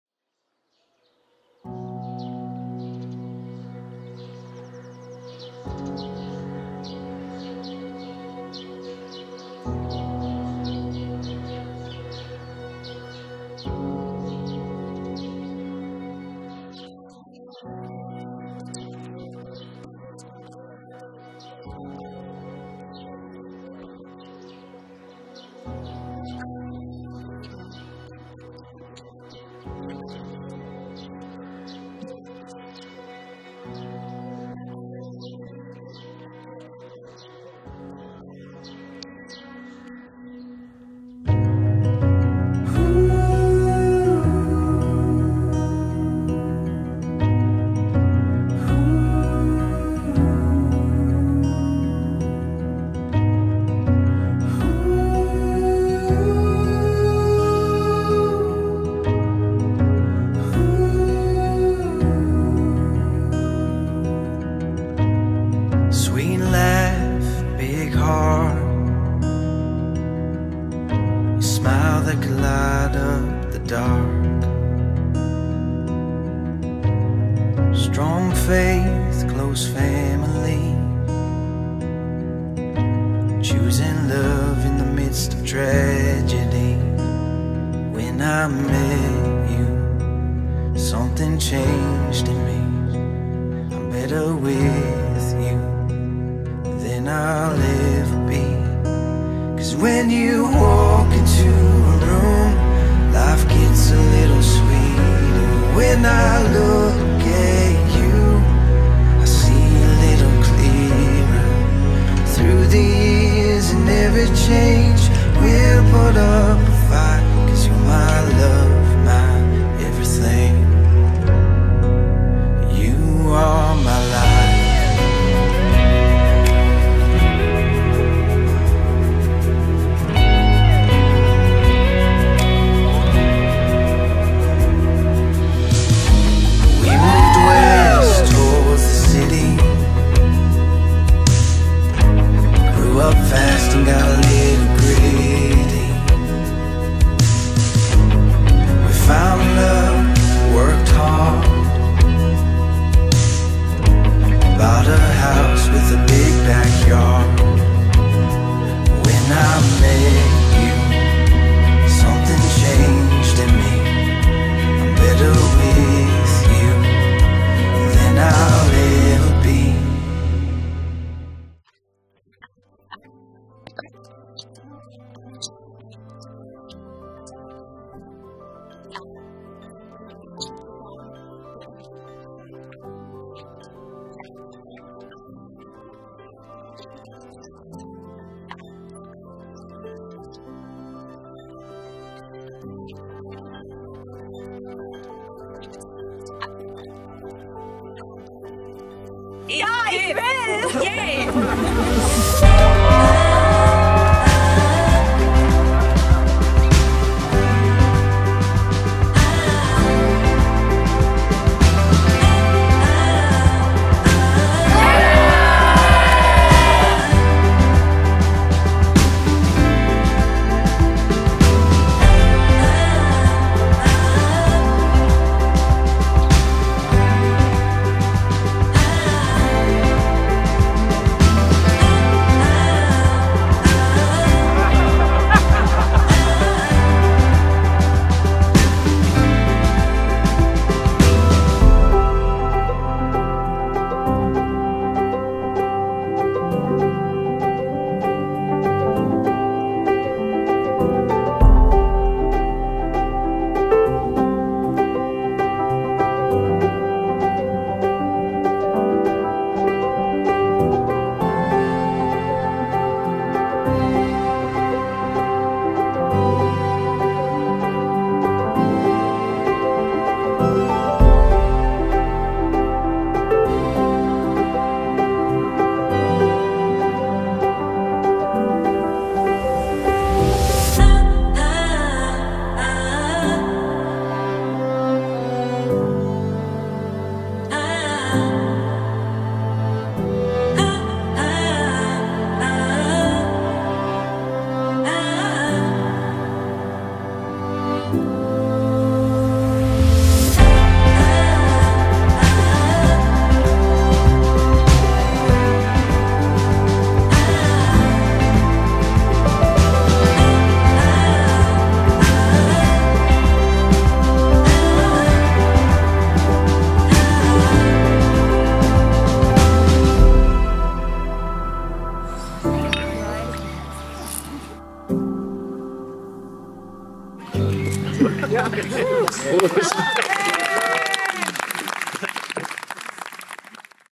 Hochzeitsvideo im Schloss Dürrenmungenau | Freie Trauung | 4K
Ich habe Sascha und Sarah bei ihrer wunderschönen Hochzeit im Schloss Dürrenmungenau begleitet. Es war eine emotionale freie Trauung.